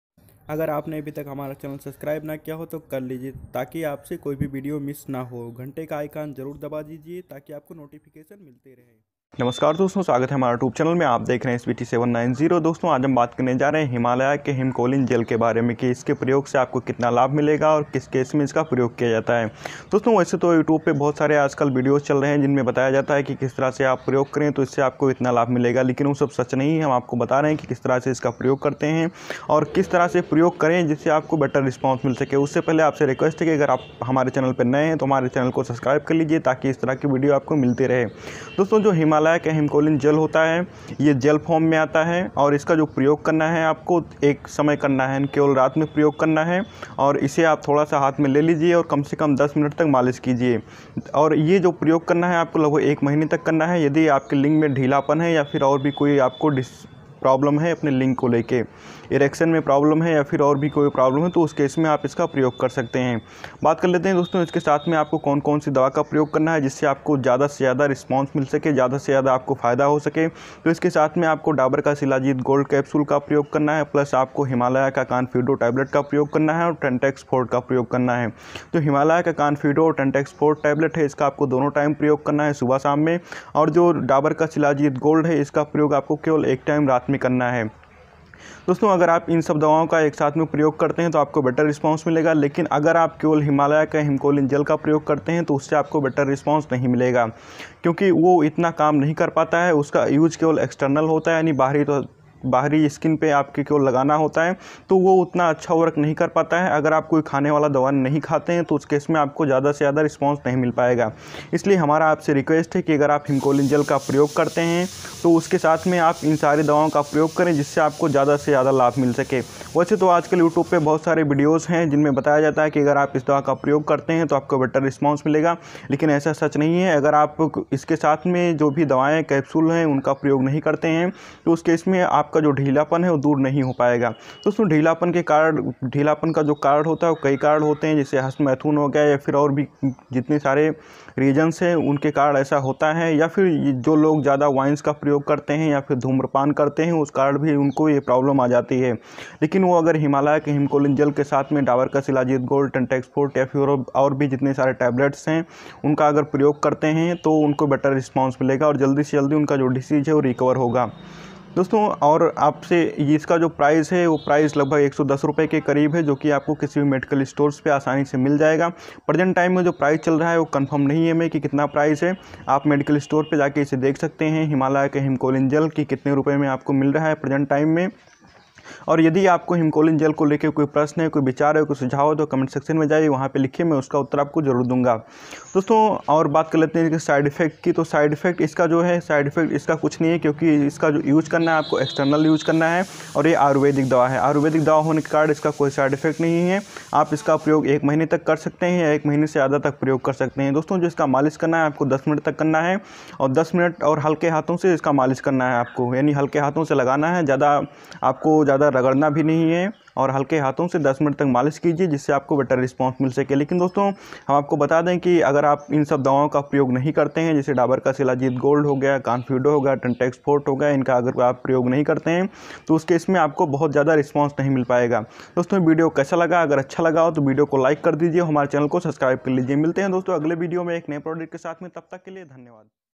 Himcolin Gel Use In Hindi | Benefits & Side Effects In Hindi
Topics covered:-
00:02 Himcolin gel in tamil,
00:15 Himcolin gel benefits,
00:50 himalaya himcolin gel side effects,
01:15 himcolin gel price,
01:30How to use Himalaya Himcolin gel,
02:00 Himcolingel,

Namskaar doston,
Himcolin gel in English,

Aaj hm baat karne ja rhe hai ki ydi aapko is tarah ki koi problem hai jaise dheelapan ya aur bhi problem hai to aap is Himalaya Himcolin Gel ka paryog kar sakte hai ,
Aur iske saath me aapko Dabur ka shilajit gold capsule aur himalaya ka confido tablet aur Himalaya ka Tentex Forte tablet ka bhi paryog karna hai ,
Agar aap in sab dawaon ka ek saath me paryog karte hai to aapko jyada laabh milega,
shilajit gold ,
shilajit gold,

Dr.Aashish shrivastava